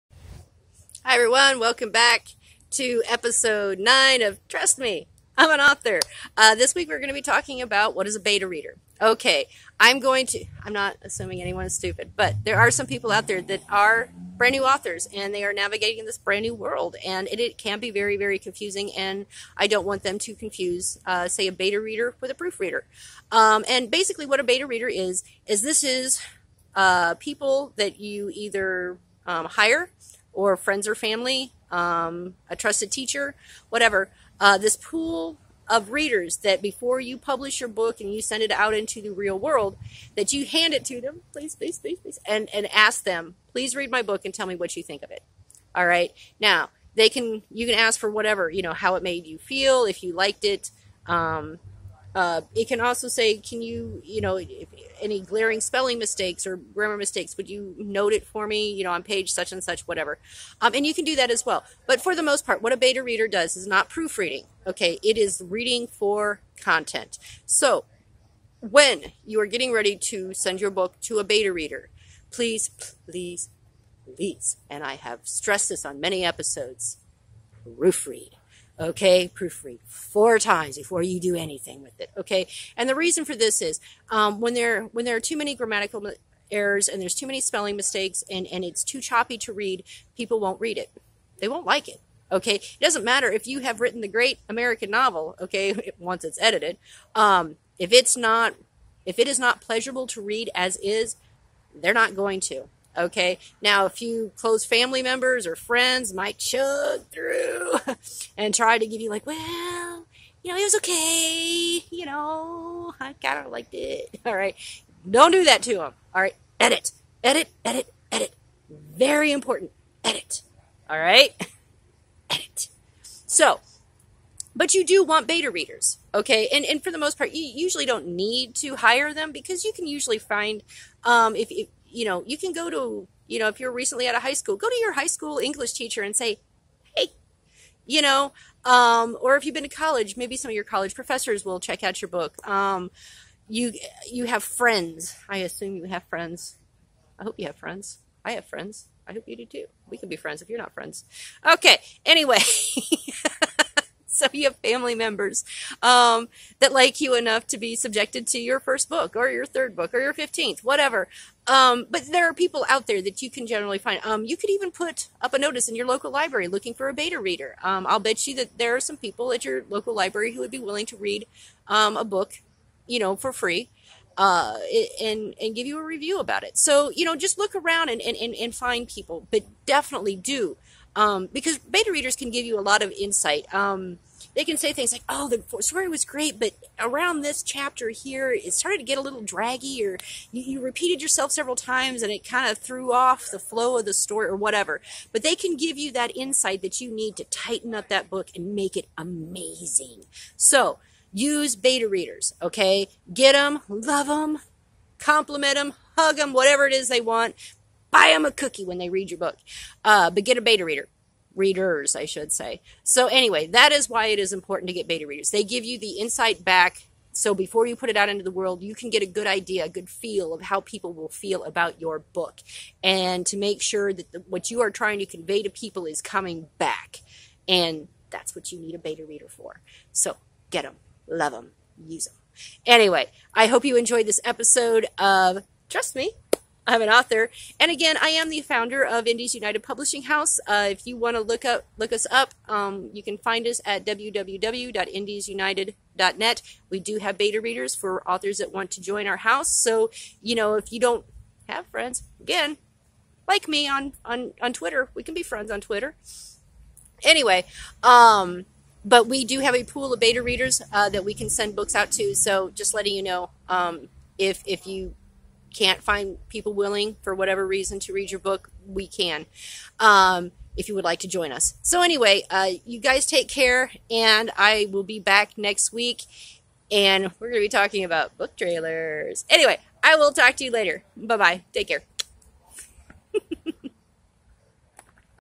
Trust Me, I'm an Author | Episode 9
Episode 9: What is a Beta Reader?
We’re talking about everything authors should know about the writing world, from editing to book trailers in this series, Trust me, I’m an Author.